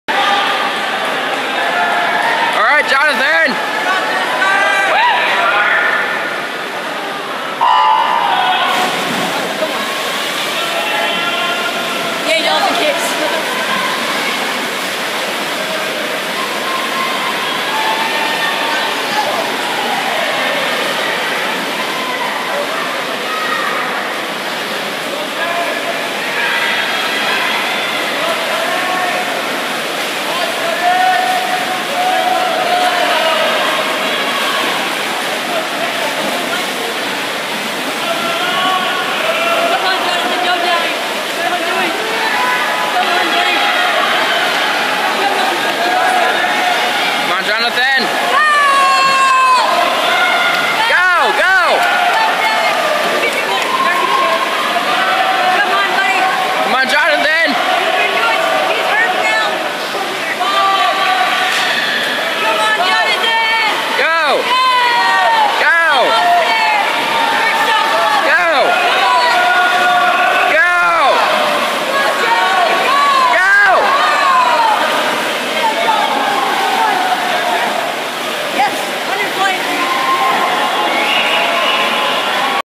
12/14/13 LPA Boys 100 Fly Johnathan
Johnathan swims his first 100 fly with a strong time of 1:10.34